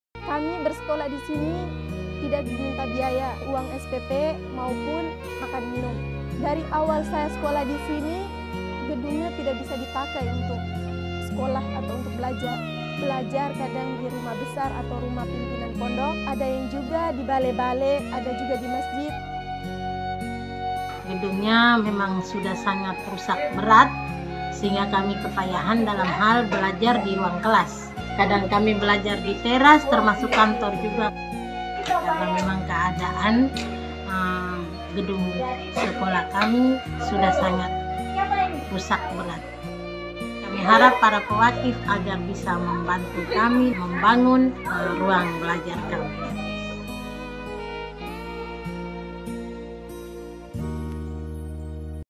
Gedung sekolah rusak berat, santri Ikhwatul Mukminin rawan tertimpa bangunan
Pesantren Ikhwatul Mukminin yang berada di Desa Adonara, Kec. Adonara, Kab. Flores Timur, NTT menjadi sekolah dambaan anak-anak dari keluarga prasejahtera/dhuafa. Santri dididik dan menimba ilmu di sini tanpa dipungut biaya.

Saat ini, bangunan pesantren rusak berat. Bagian atapnya bocor di mana-mana. Dindingnya retak-retak, kayu-kayu penyangga atap juga sudah rapuh. Bukan cuma tidak nyaman, kondisi ini jelas sangat membahayakan. Sayangnya, pesantren tak mampu meremajakan bangunannya karena terkendala biaya.

Pesantren Ikhwatul Mukminin membutuhkan Rp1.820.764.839 untuk membangun gedung baru seluas 13x18 m² di atas lahan gedung yang lama. Gedung akan dibuat 2 lantai, dilengkapi dengan 6 ruang kelas, toilet santri, 60 pasang meja dan kursi, serta fasilitas penyokong lainnya.

Ayo, bantu santri Ikhwatul Mukminin miliki sekolah yang aman dan layak dengan berwakaf di

Transfer wakafmu ke sini:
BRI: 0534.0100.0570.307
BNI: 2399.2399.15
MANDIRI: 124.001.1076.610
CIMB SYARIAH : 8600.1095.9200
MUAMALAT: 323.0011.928

Sertakan infak kode unik 516 di belakang nominal. Contoh: Rp100.516